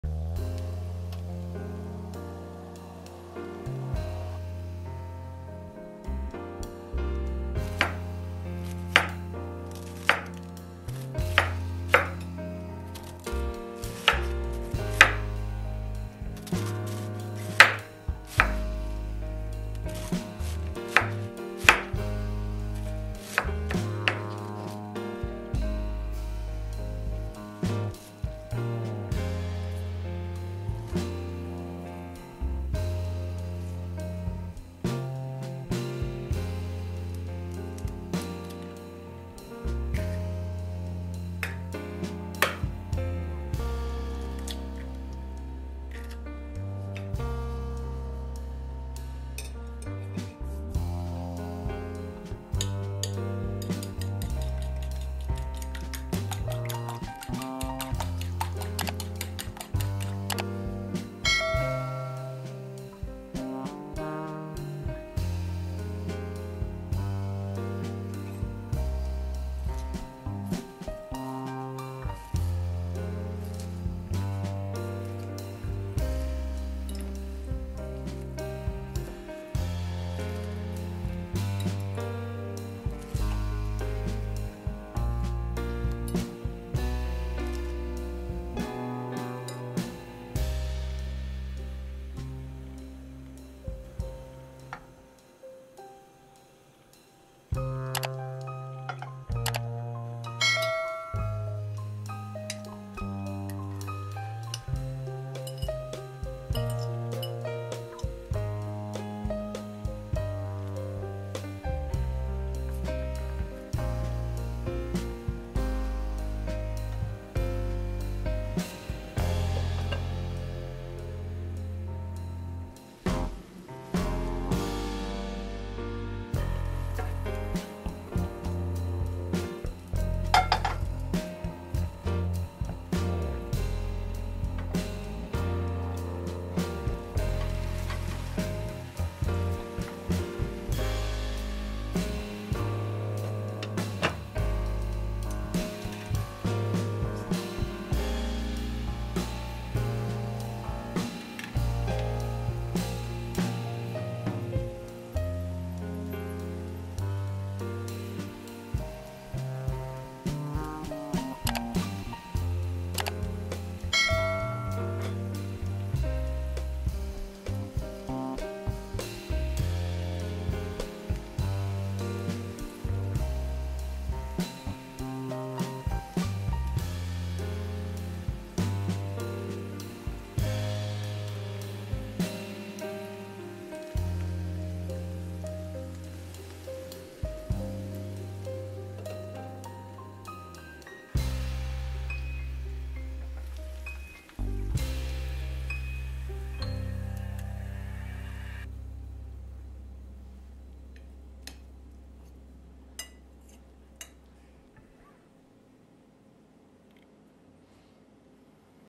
Cabbage with eggs is better than pizza! Quick, simple and delicious!
Delicious, easy, quick and very delicious breakfast, lunch, dinner or quick snack recipe. You won't believe how tasty it is – even more delicious than pizza, or okay definitely healthier 😅 It's so easy to make, and you'll enjoy every step of the cooking process. Try this easy and very delicious dish and let me know in a comments "How do you like it?"

INGREDIENTS:
1/2 small cabbage (250 g)
2 eggs
Season with salt
1 tbsp oil
2 tbsp pizza sauce
mozzarella
basil
Subtitles are available in different languages. Please select your language in the settings
Thank you for the comments and likes!